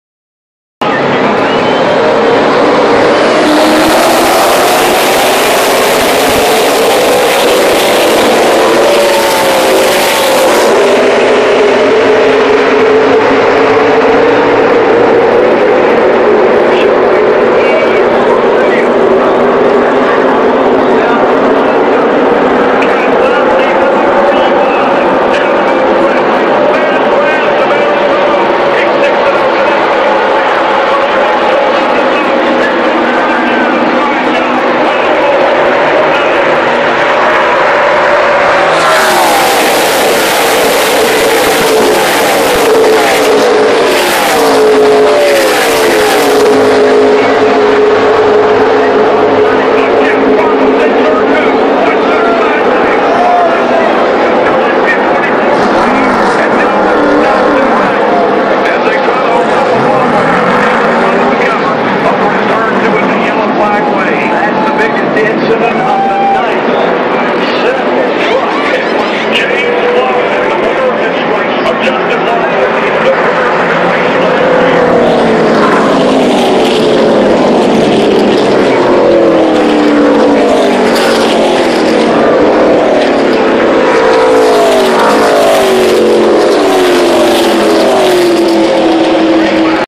NASCAR Truck Race in Charlotte late restart
Late restart and spin in the History 200 Camping World Truck Series at Charlotte Motor Speedway.